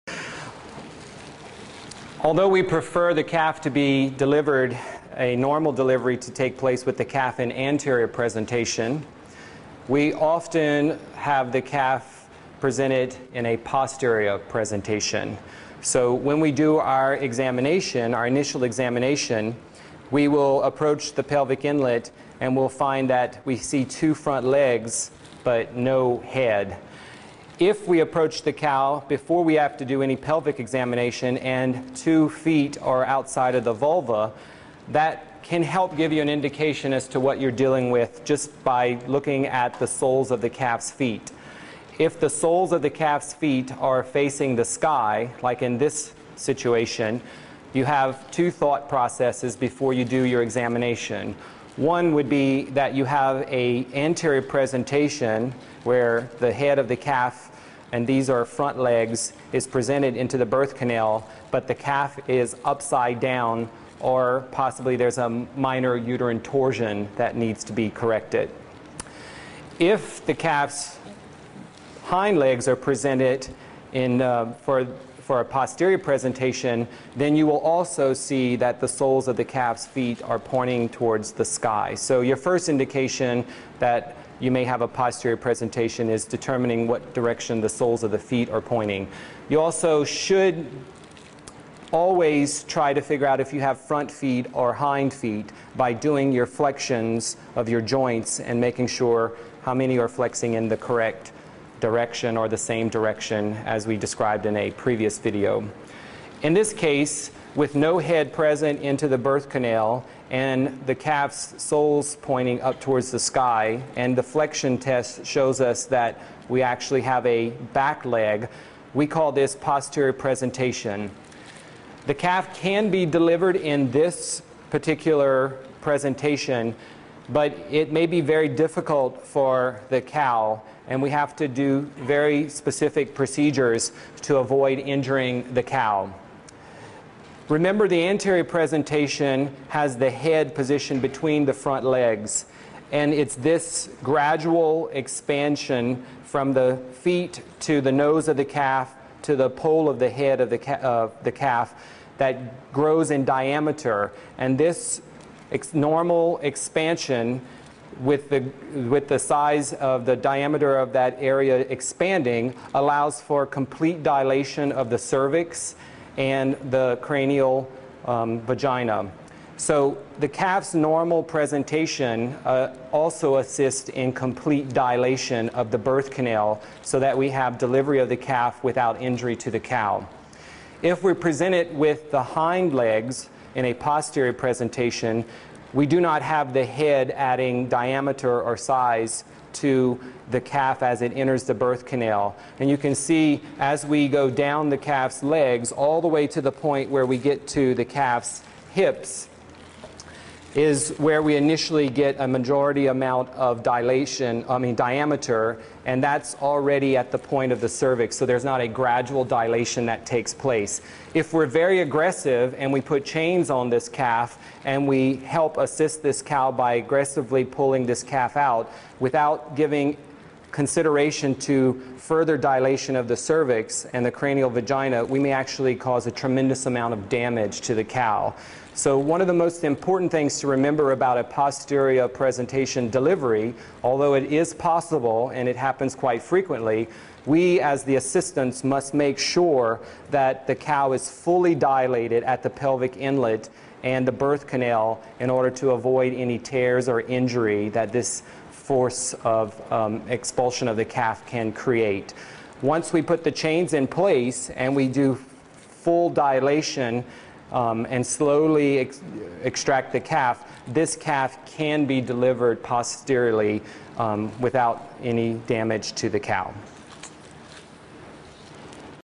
Posterior Presentation Dystocia
Dr. Billy Smith explains a posterior presentation dystocia.  Dystocia, more commonly known as difficult calving, is a problem encountered by dairy farmers.